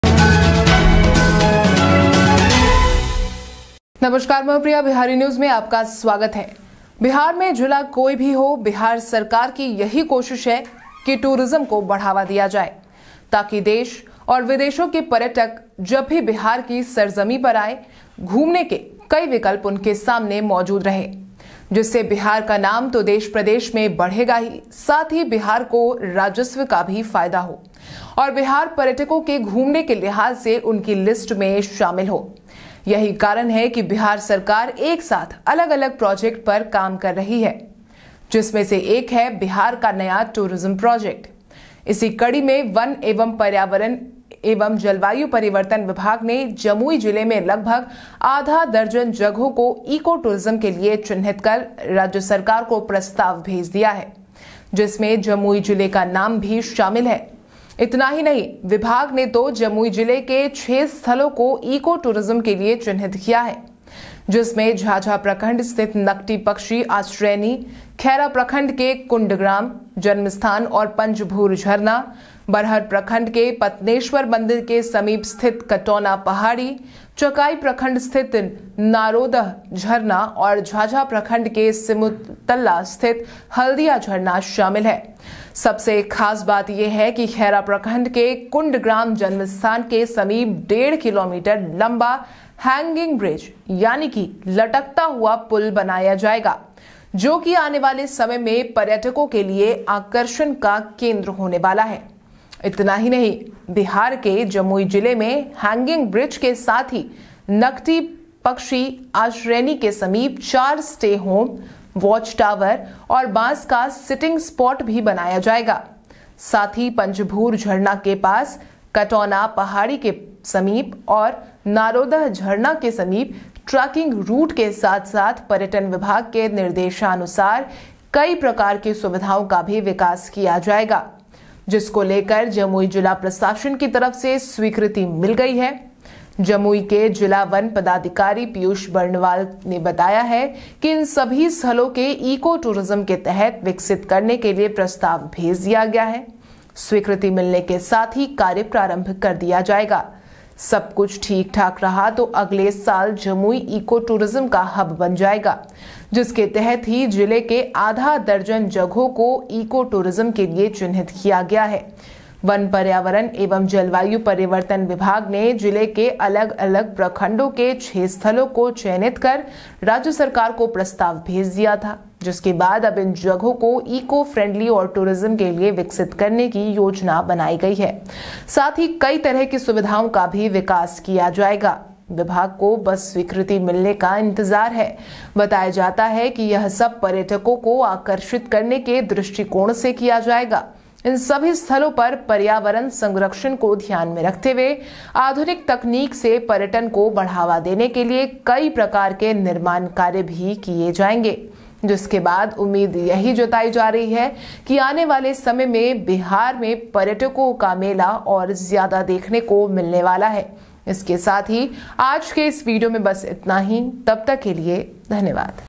बिहार के इस जिले में बनेगा 1.5 किलोमीटर का लंबा हैंगिंग ब्रिज, पर्यटन को मिलेगा बढ़ावा
Be it the district in Bihar, it is the endeavor of the Bihar government to promote tourism so that whenever the tourists of the country and abroad come to the soil of Bihar, many options are available in front of them, so that the name of Bihar will increase in the country. Bihar will also benefit from revenue and Bihar will be included in their list for tourists to visit, this is the reason why Bihar government is working on different projects together, one of which is Bihar's new tourism project, in this episode. The Forest and Environment and Climate Change Department has identified about half a dozen places in Jamui district for eco-tourism and sent a proposal to the state government. In which the name of Jamui district is also included, not only this, the department has also identified six places of Jamui district for eco-tourism.